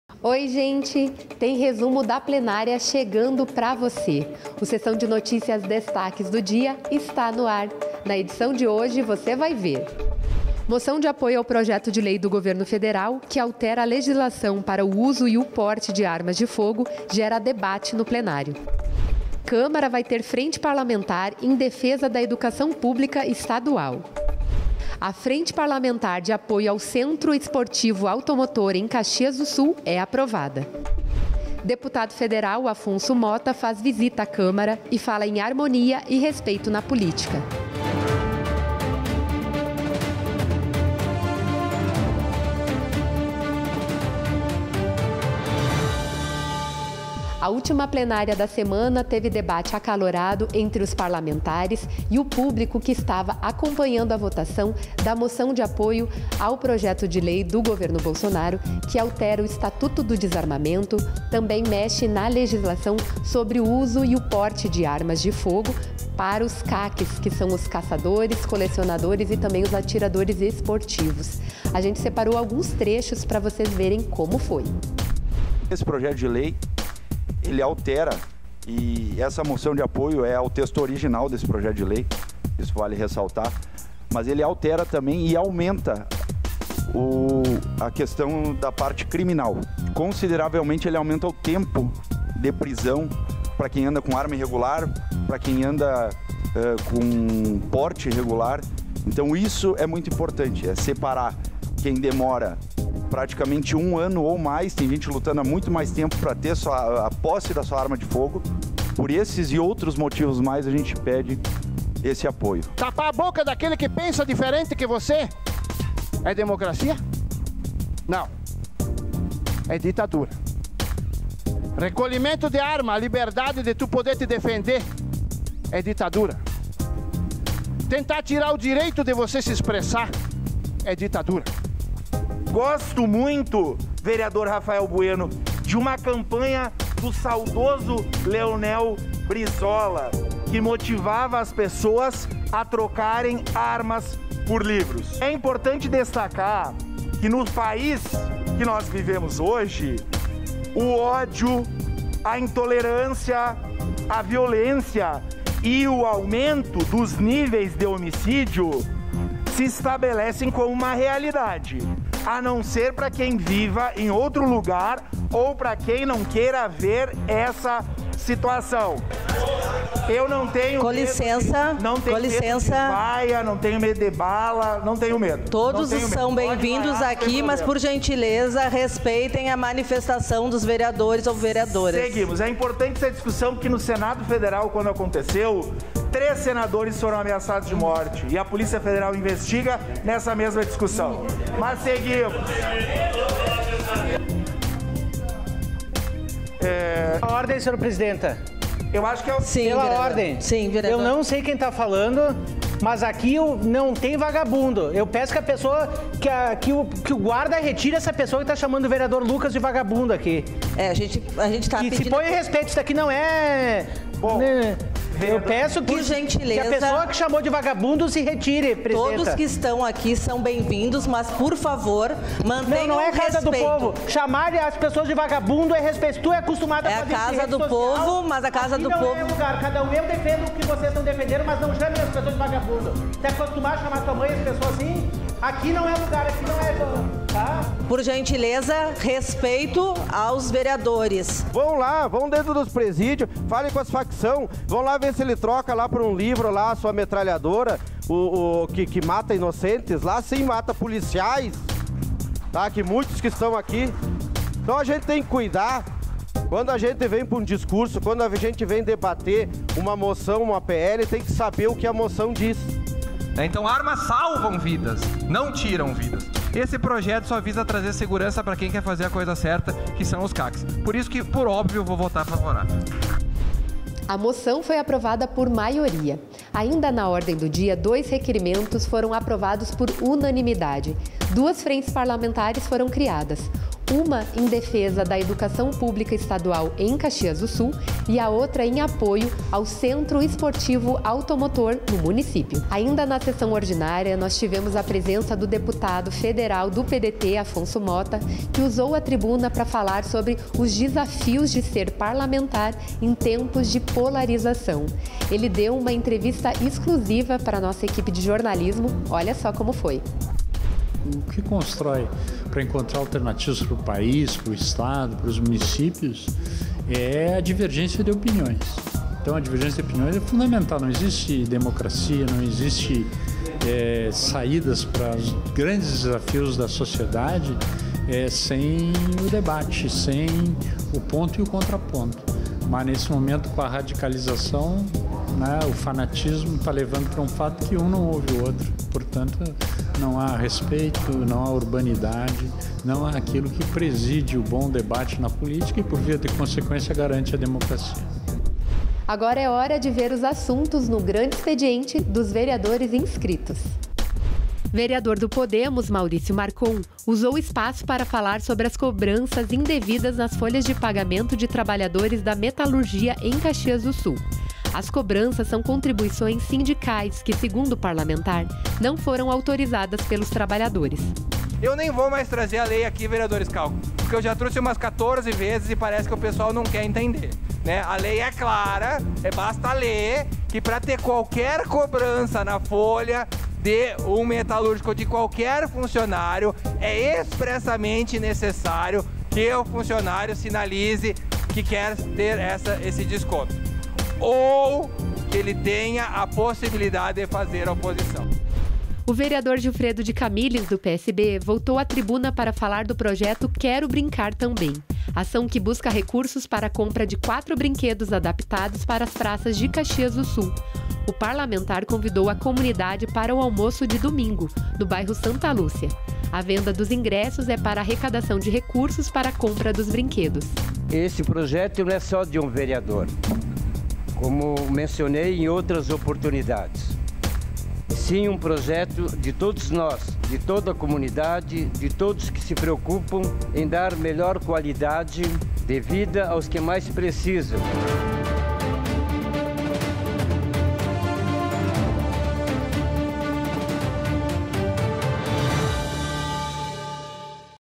Moção sobre porte de armas gera debate | 07.04.2022
Oi gente. Resumo da plenária de hoje chegando pra você :) Sessão de Notícias destaquesdodia está no ar! Na edição de hoje você vai ver

- Moção de apoio ao PL do governo federal, que altera a legislação para o uso e o porte de armas de fogo, gera debate no plenário;

- Câmara vai ter Frente Parlamentar em Defesa da Educação Pública Estadual;
- A Frente Parlamentar Pró Centro Esportivo Automotor em Caxias do Sul é aprovada;
- Deputado federal, Afonso Motta, faz visita a Câmara e fala em harmonia e respeito na política. Fique com a gente ;)